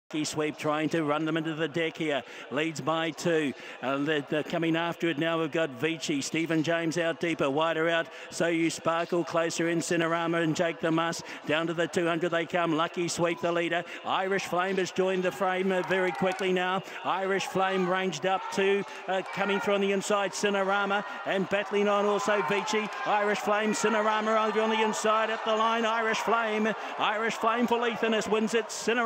Race Replay: Ellerslie Race 5 - 9 March
Fasttrack Insurance 1600

1st: Irish Flame (Leith Innes)
2nd: Sinarahma (Johnathan Parkes)
3rd: Lucky Sweep (Sam Spratt)